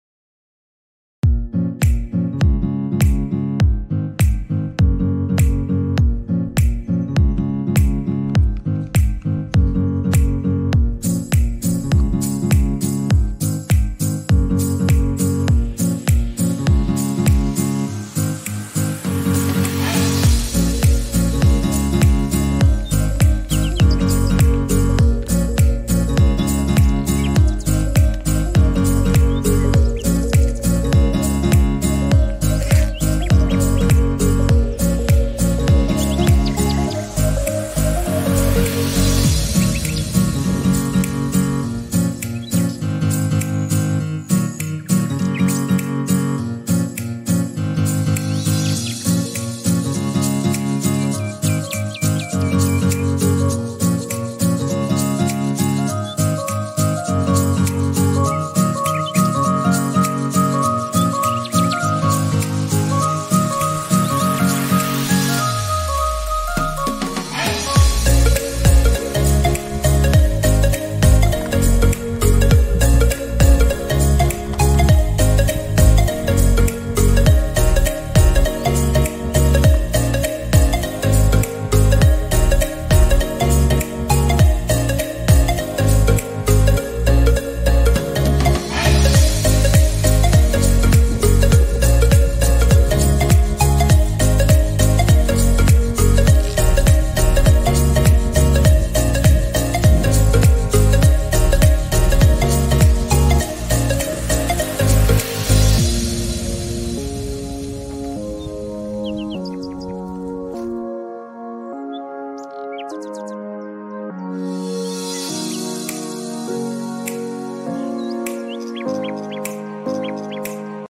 TUTO DE POSE PIEGE PROCEREX
N'hésitez pas à consulter la vidéo pour faciliter la pose de votre piège PROCEREX